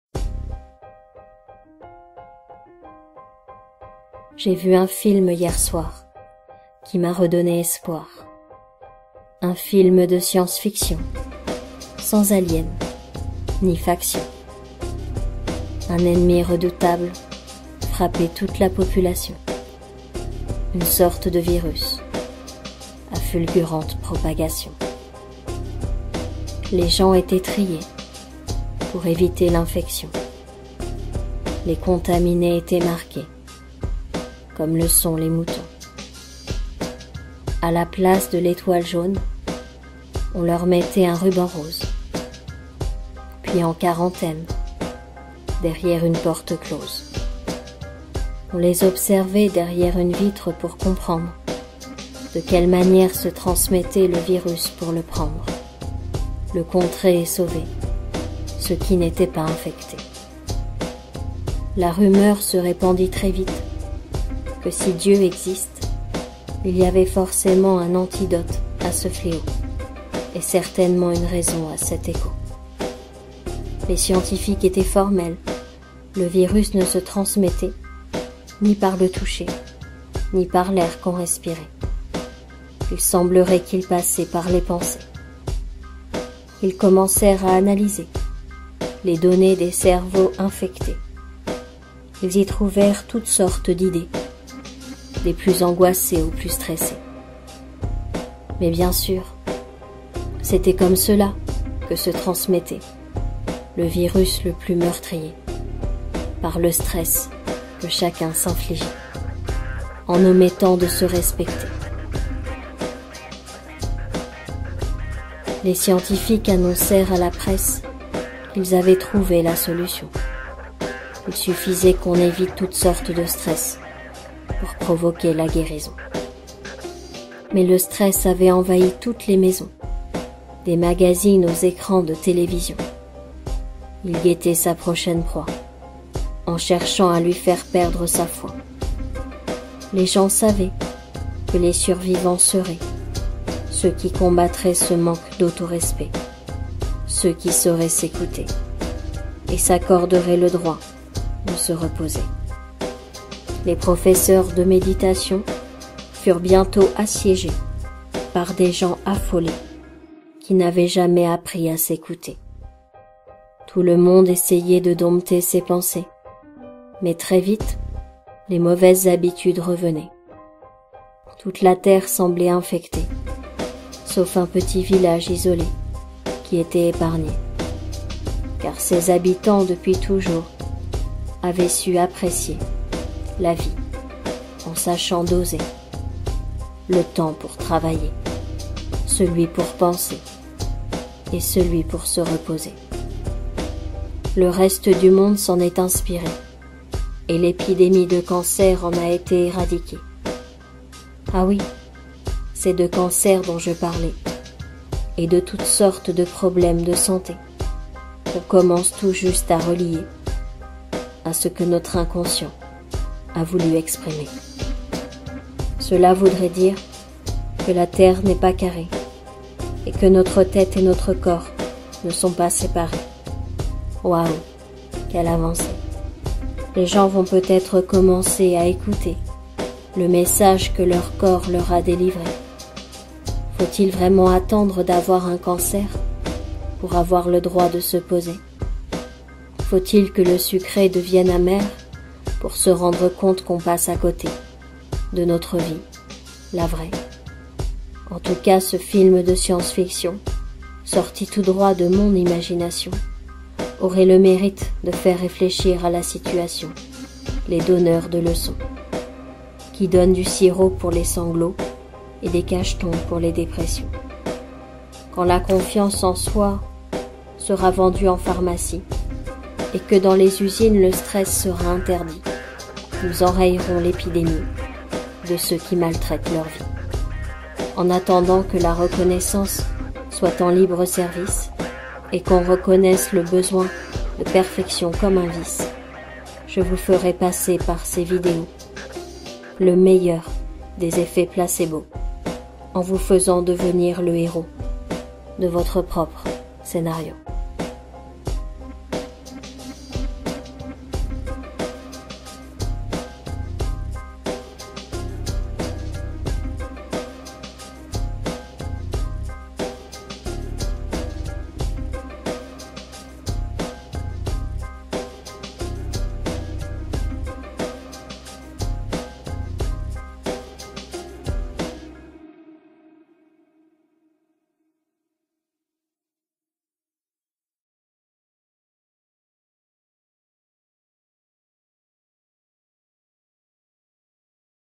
Stress et Santé by Camille Griselin HYPNOSE
Séance d'hypnose pour vaincre le stress et éviter les répercussions du stress sur la santé